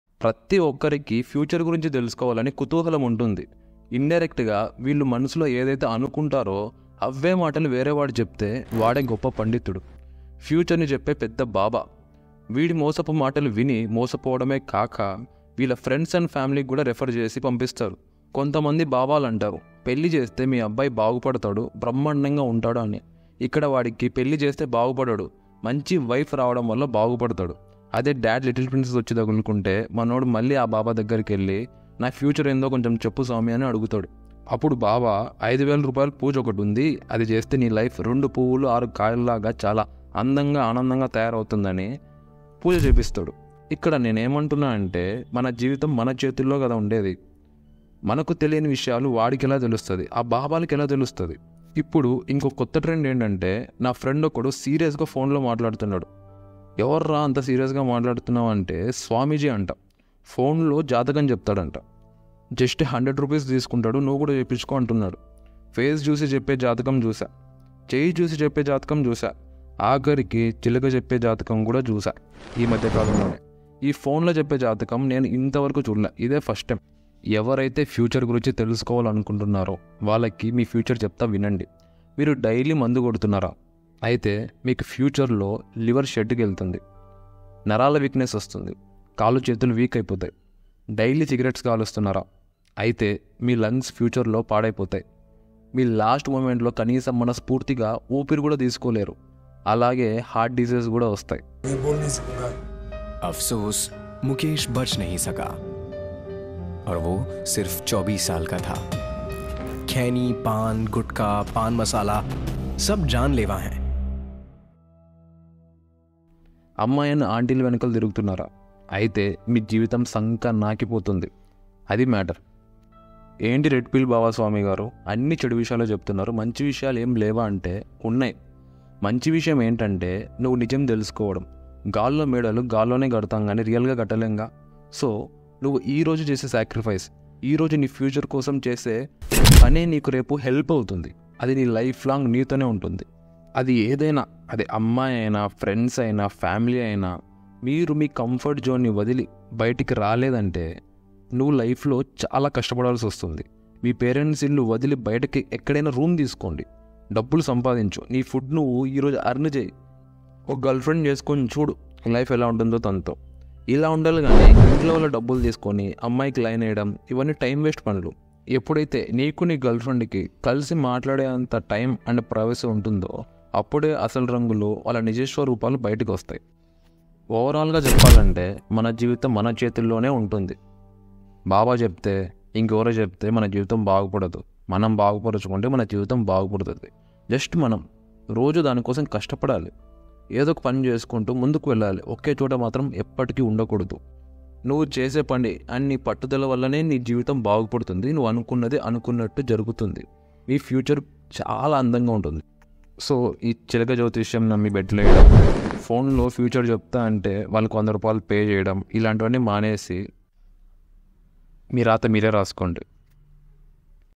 Truth about Horoscopes | How Astrology Fools Millions of Indians! | Red Pill Telugu
Astrology is a widely practiced phenomenon, especially in India. However, the debate surrounding its authenticity is even more popular. Is astrology genuine or merely an illusion? How do some individuals manage to make accurate predictions? Is it a form of superpower? And what about horoscopes? Are they real?

In this enlightening video by Dhruv Rathee, we delve into the reasons why astrology is deceptive. Dhruv discusses various aspects and theories, shedding light on why astrology fails to stand up to scrutiny. If you've ever wondered about the truth behind horoscopes and astrology, this video provides valuable insights.